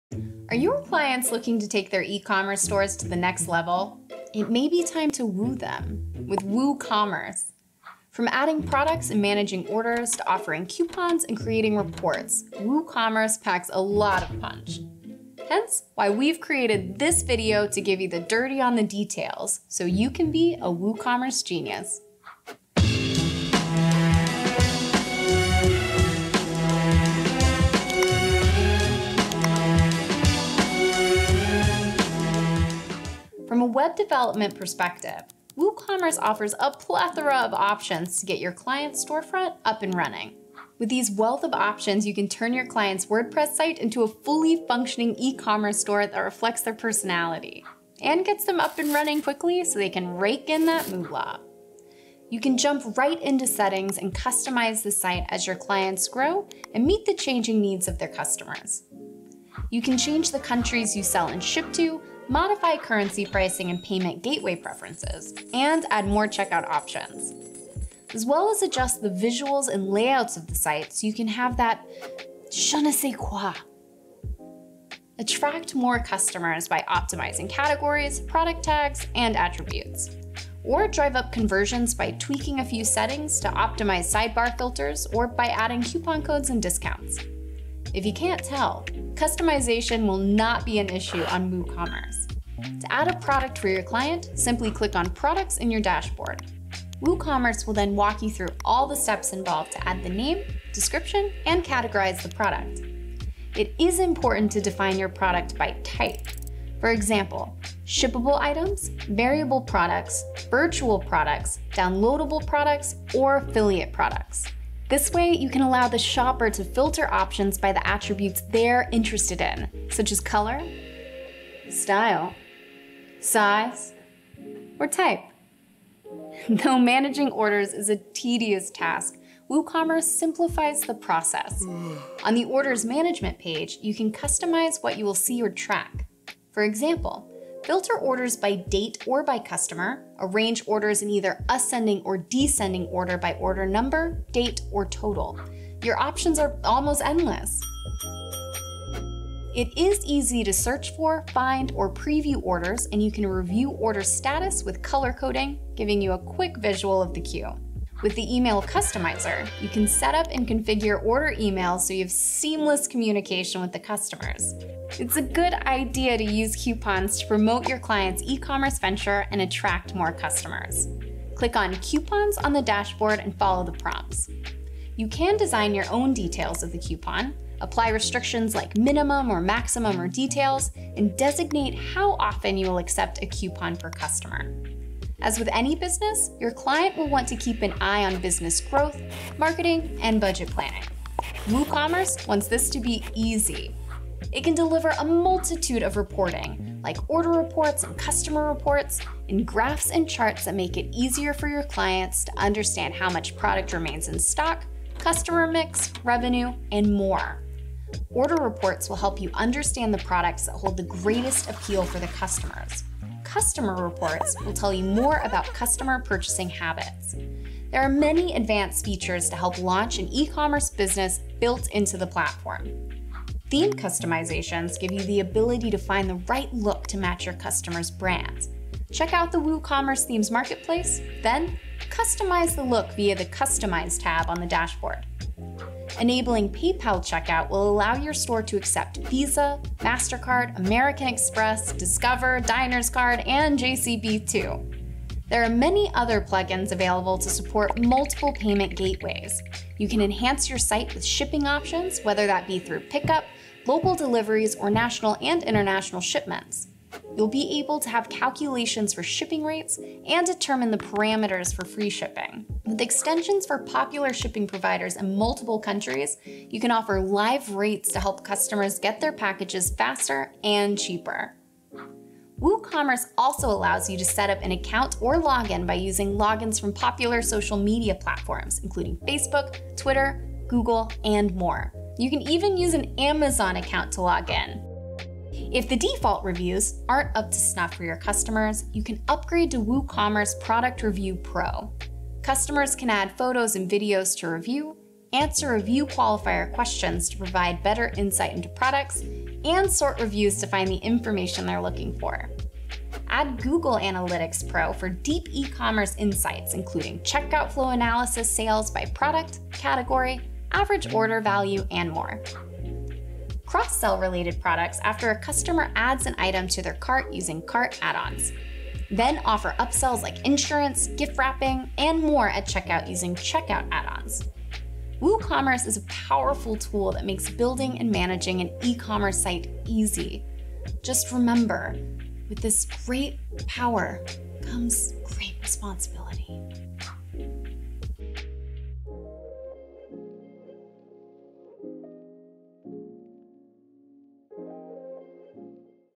2022 WooCommerce Tutorial - How to use WooCommerce
Here are some WooCommerce basics to create an online store. Learn how to use WooCommerce with GoDaddy’s tutorial for beginners.

We’ll discuss the basics of managing WooCommerce settings, products, orders and more to successfully launch an online store.

00:00 – How to use WooCommerce
00:52 –  Managing WooCommerce settings
01:34 – Adding a product
02:08 – Managing orders
02:47 – Creating coupons
03:06 – Understanding reports
03:41 – Advanced WooCommerce features

Made for website designers and developers, the Hub makes it easy to maintain multiple sites with updates, backups, security scans and performance checks